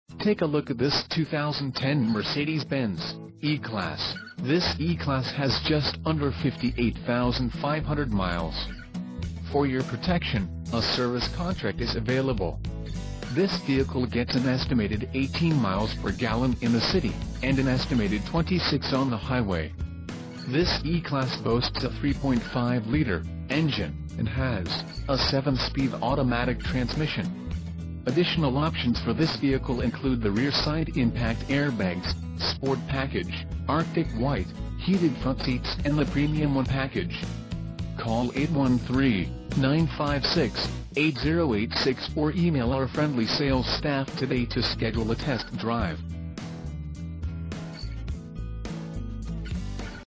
2010 Mercedes-Benz E-Class 4dr Sdn E350 Sport RWD Sedan - Tampa, FL
Tampa Bay Auto Network, Tampa, FL - ~ NAVIGATION ~ SPORT PACKAGE ~ P1 ~ HEATED SEATS ~ SUNROOF ~ LOGIC7 ~ SUNSHADE ~